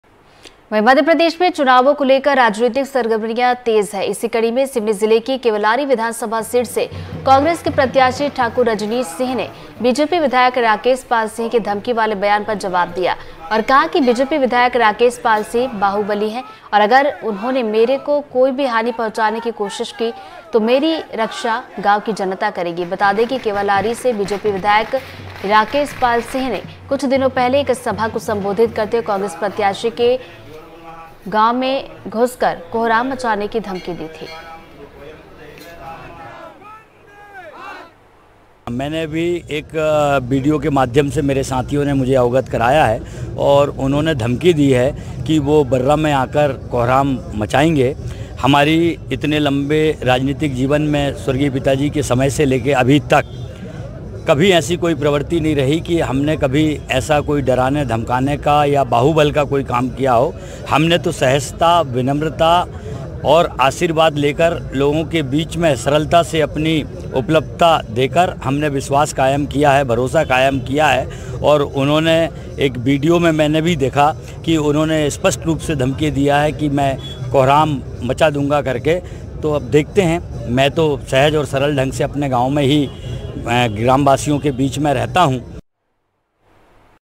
Keolari से Congress Candidate Thakur Rajneesh Singh ने BJP MLA Rakesh Pal Singh की धमकी पर क्या कहा?
मध्य प्रदेश (Madhya Pradesh) में चुनावों को लेकर राजनीतिक सरगर्मियां तेज हो गई है. सिवनी (Seoni) जिले की केवलारी (Keolari) विधानसभा सीट से कांग्रेस प्रत्याशी ठाकुर रजनीश सिंह (Thakur Rajneesh Singh) ने BJP विधायक राकेश पाल सिंह (Rakesh) के धमकी वाले बयान पर जवाब दिया और कहा कि BJP विधायक राकेश पाल सिंह बाहुबाली हैं और अगर उनहोंने मेरे को कोई भी हानि पहुंचाने की कोशिश की तो मेरी रक्षा गांव की जनता करेगी.